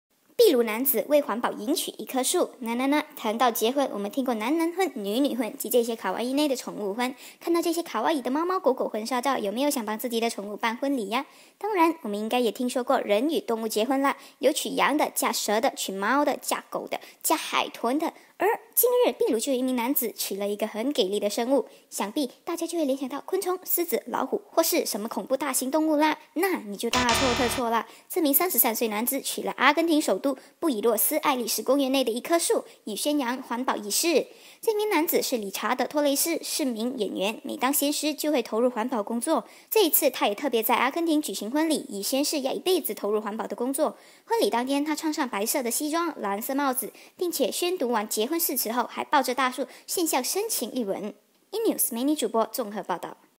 [Enews 美女主播] 秘鲁男子为环保迎娶一棵树
谈到结婚 我们听过男男婚 女女婚 及这些卡哇伊呐的 宠物婚
看看这些卡哇伊的猫猫狗狗婚纱照 有木有想帮自己的宠物办婚礼啊
当然 我们应该也听说过人与动物结婚啦 有娶羊的 嫁蛇的 娶猫的 嫁狗的 嫁海豚的
而近日 秘鲁就有一名男子 娶了一个很给力的生物
想必大家就会联想到 昆虫 狮子 老虎 或是 什么恐怖大型动物
那你就大错特错啦 这名33岁男子是娶了阿根廷首都 布宜諾斯艾利斯公園內的一棵樹
以宣揚環保意識

这名男子是理查德·托雷斯 是名演员 每当闲时就会投入环保工作
这次他也特别在阿根廷舉行"婚禮" 以宣示要一輩子投入環保的決心
婚礼当天他穿上白色西装 蓝色帽子 并且宣讀完結婚誓詞后 还抱著大樹獻上深情一吻